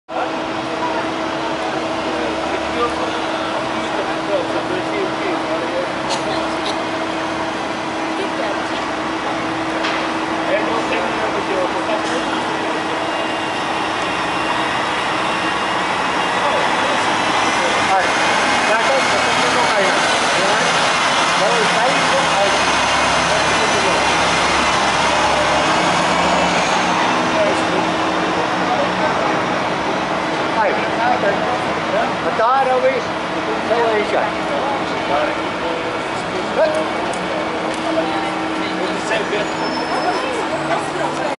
Koningsdag voorbereidingen Dam
Wat een megaklus. En wij kregen vandaag bericht dat maandag het centrum praktisch afgesloten zal zijn voor auto's. In ieder geval wordt het een puinhoop en 's avonds mag het losbranden.